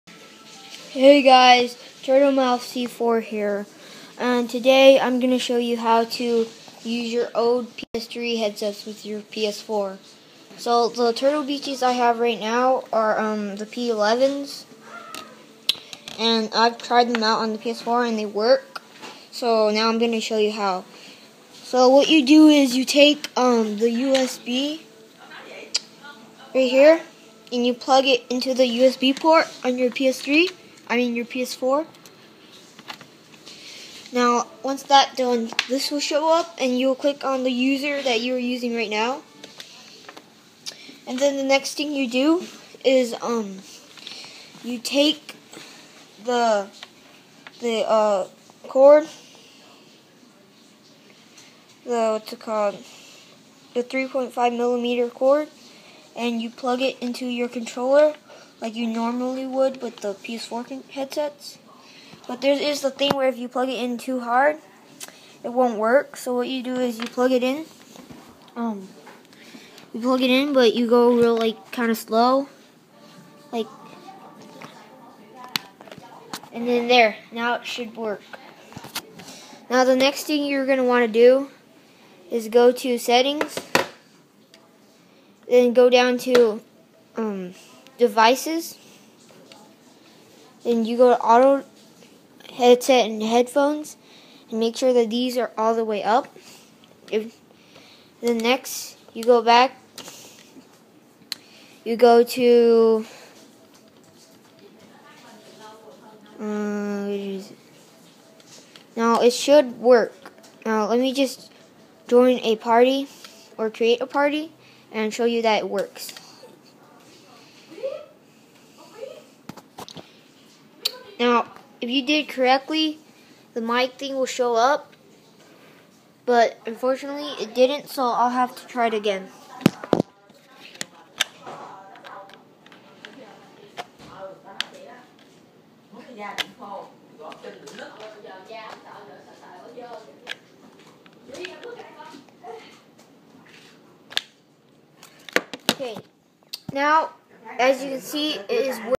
How to use ps3 headsets with ps4
Basically you plug the USB into the USB port and plug the 3.5 mm into the controller